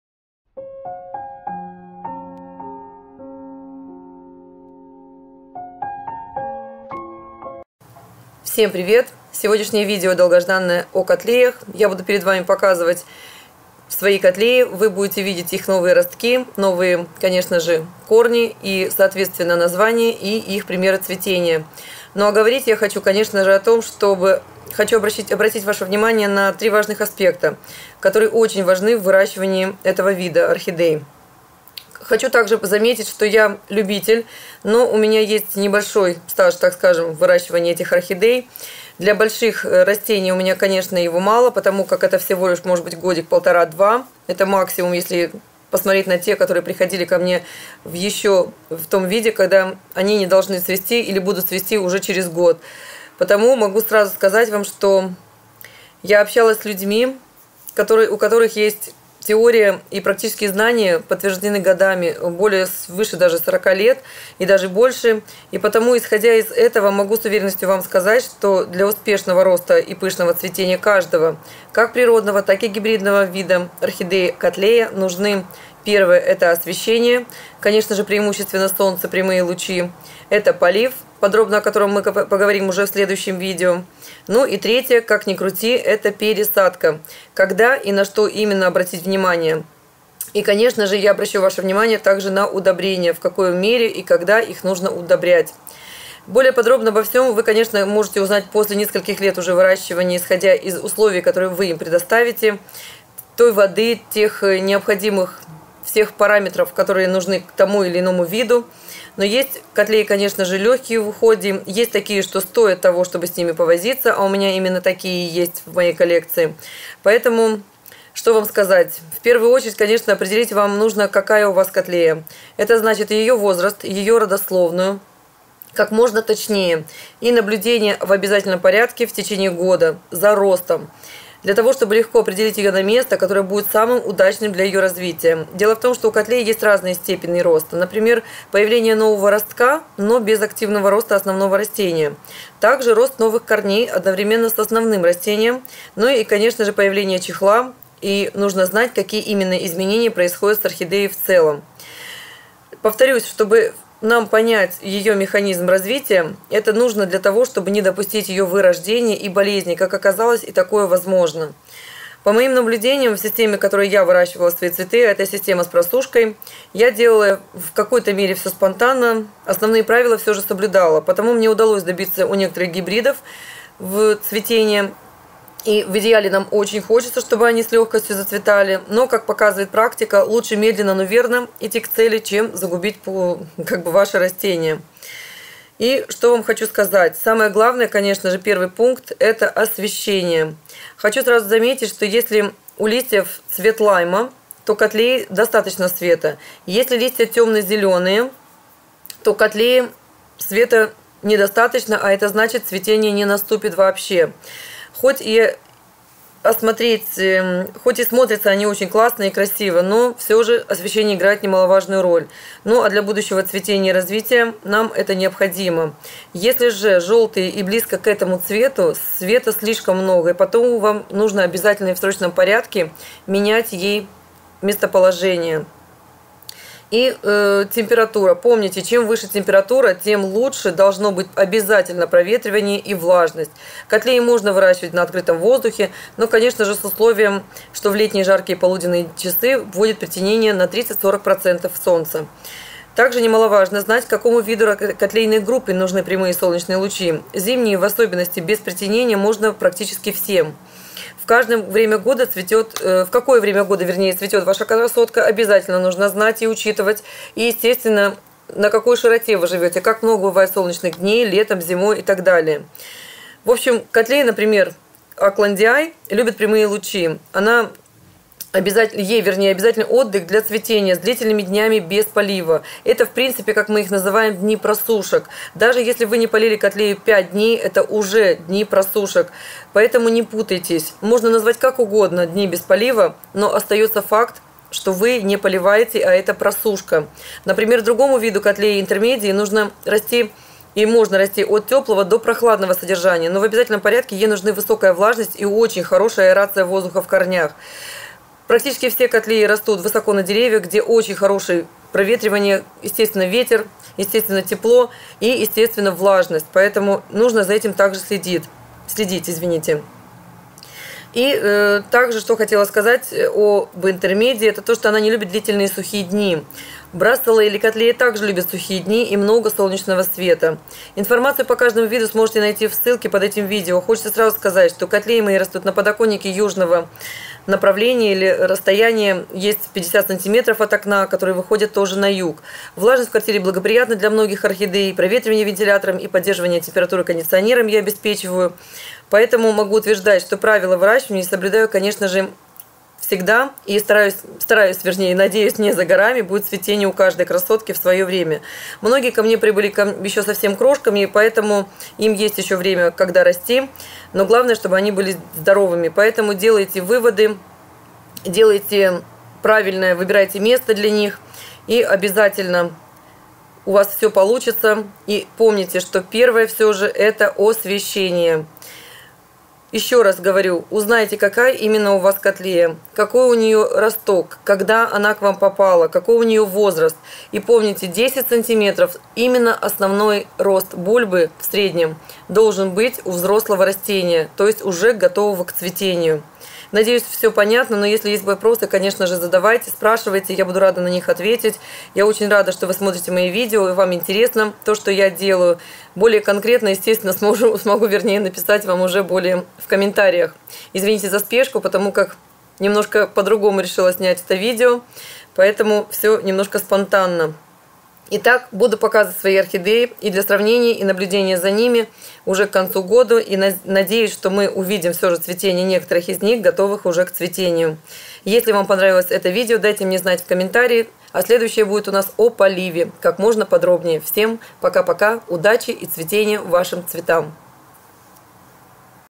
Каттлее нужен солнечный свет) ч.1, the main thing for cattleya is sunshine!
Всем привет! Показываю свои каттлеи и рассказываю что именно нужно для их роста и развития. 1. Освещение (преимущественно солнце, прямые лучи); 2. Полив (подробно в следующем видео); 3. Пересадка ( когда и на что обратить внимание, удобрения). Более подробно обо всём только вы сможете знать после нескольких лет выращивания, исходя из условий, которые вы им предоставите. Есть каттлеи очень лёгкие в уходе, а есть такие, что стоят того, чтобы с ними повозиться. В первую очередь определите какая у вас каттлея (возраст, название, как можно точнее, наблюдения в течении года за ростом). Для того чтобы легко определить её на место, которое будет самым удачным для развития. Дело в том, у каттлей есть разные степени роста, например, появление нового ростка, но без активного роста основного растения, также рост новых корней одновременно с основным растением, и также когда появляется чехол, какие именно изменения происходят с орхидеей в целом. Всё это нужно для того, повторюсь, чтобы понять механизм её развития и также для того, чтобы не допустить её вырождения и болезней. пункт ОСВЕЩЕНИЕ, хочу сразу заметить что если у листьев цвет лайма, каттлее достаточно света, если листья тёмно-зелёные, это недостаточно освещения (хоть и смотрятся они очень неплохо), для будущего цветения и развития нового ростка, но если у каттлеи листья жёлтые или близки к этому цвету, света слишком много, и потому именно местоположение относительно солнца очень важно. И помните чем выше температура, тем лучше должны быть проветривания и влажность. Информацию по каждому виду вы сможете найти в ссылке под этим видео. Хочется сразу сказать, мои каттлеи растут все на подоконнике южного окна или на расстоянии 50 см от окна, которые выходят на Юг. Влажность в квартире благоприятна для многих орхидей, проветривание вентилятором и поддержание температуры кондиционером я обеспечиваю, потому могу утверждать, все правила выращивания я соблюдаю и конечно не за горами цветение каждой красотки). . Если вам понравилось это видео, дайте мне знать в комментарии, следующее будет о поливе), как можно подробней).

Есть и другие видео за 2017-18 г., приглашаю заглянуть на канал, плейлисты.